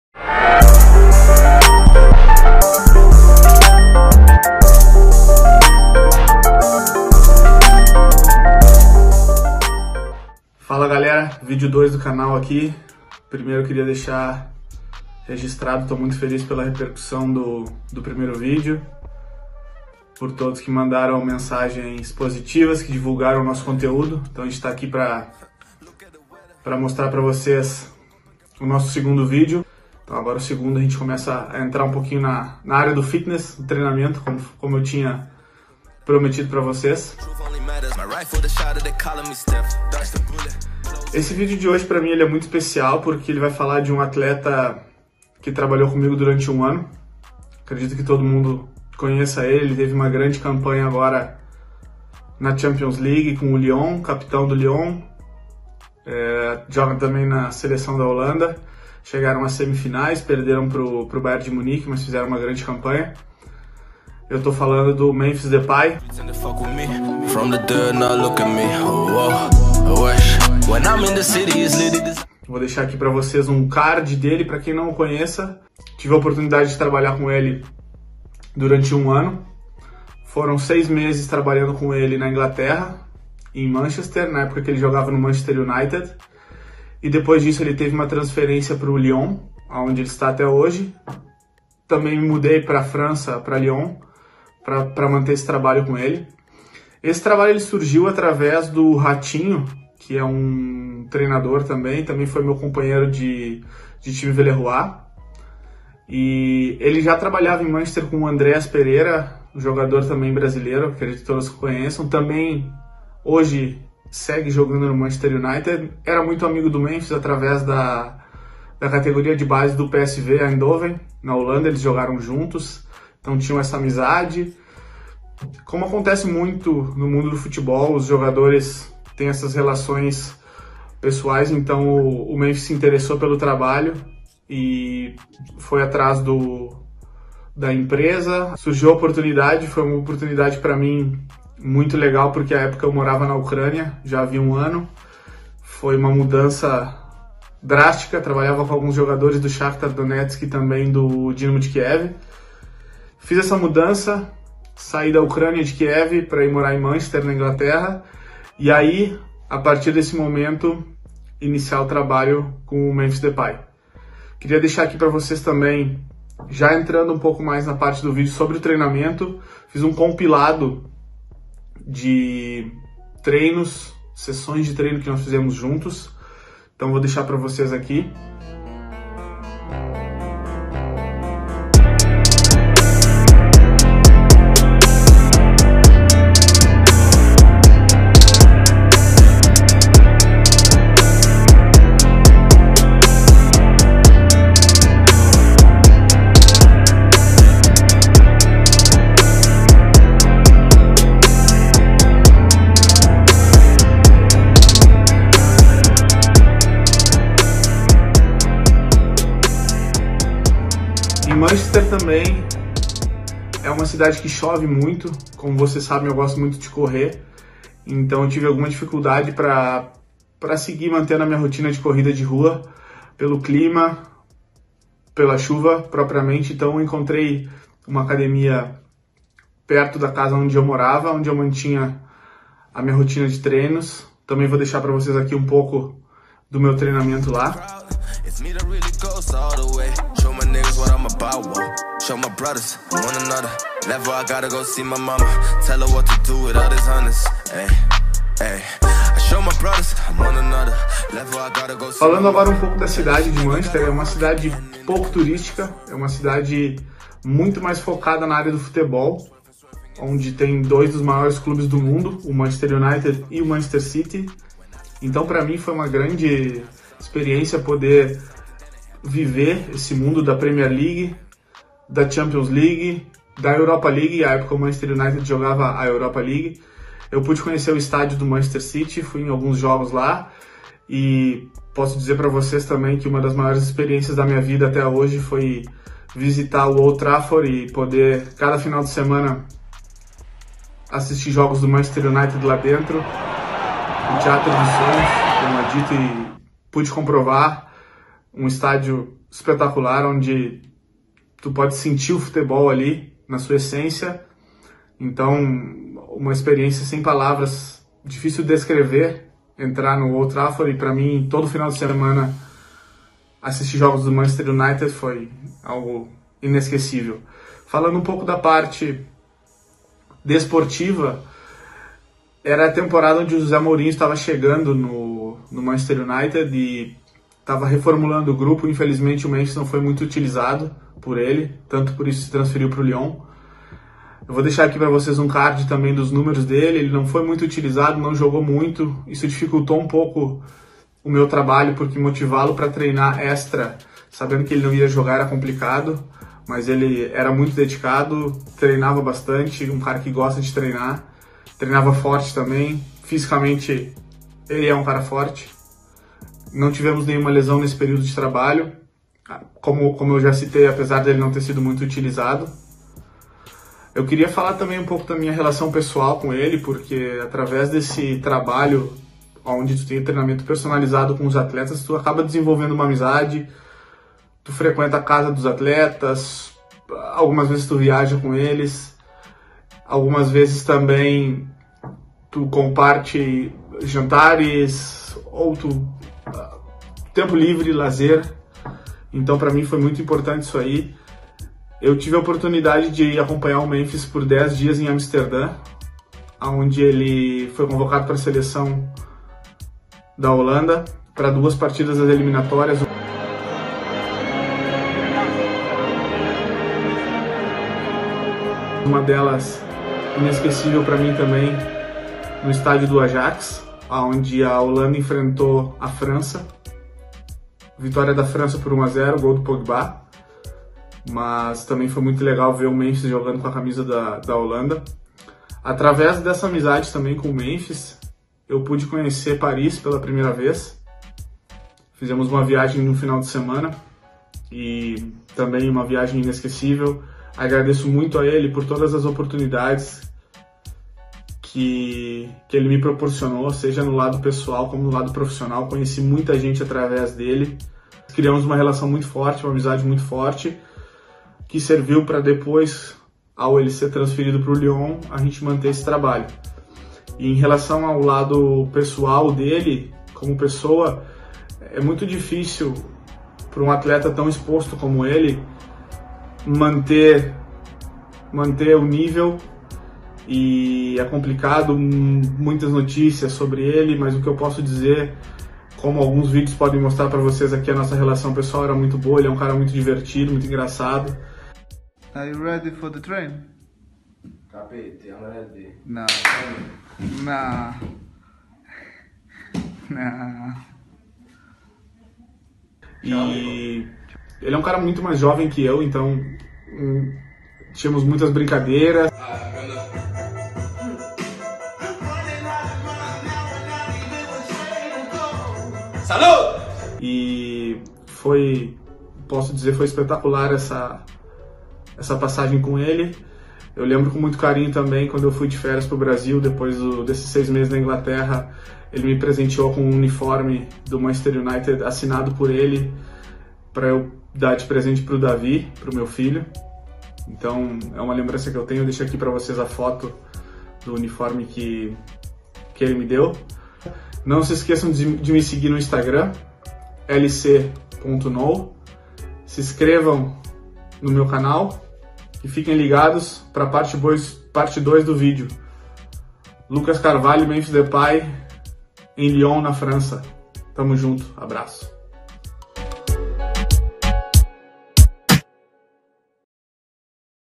Memphis Depay and LC part 1 - Workout - Lifestyle in Manchester
Uma mudança brusca, da Ucrânia para a Inglaterra, primeiro período de trabalho com o Memphis, treinamentos, jogos, a cidade, relação pessoal e muito mais! Memphis e LC parte 1 - Manchester, Inglaterra.

A drastic change, from Ukraine to England, the first period of work with Memphis, trainings, matches, the city, our personal relashionship and much more! Memphis and LC part 1 - Manchester, UK.